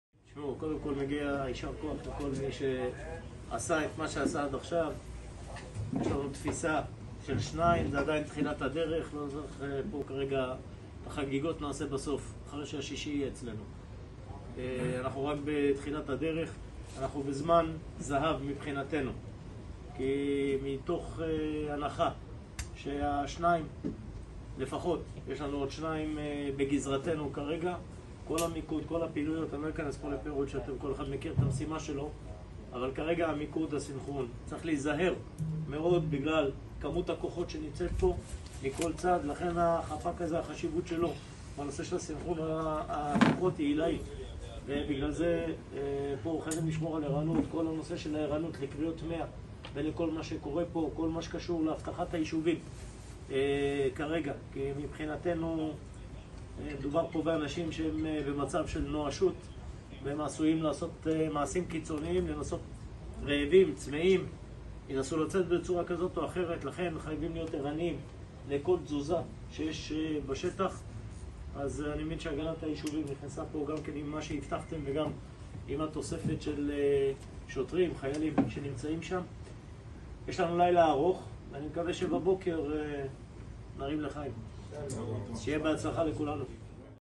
بعد القبض على اربعة من الأسرى, المفوض العام للشرطة قام بتقييم الوضع في مركز التحكم والسيطرة
بعد القبض على اربعة من الأسرى, المفوض العام للشرطة قام  بتقييم الوضع في مركز التحكم والسيطرة للشرطة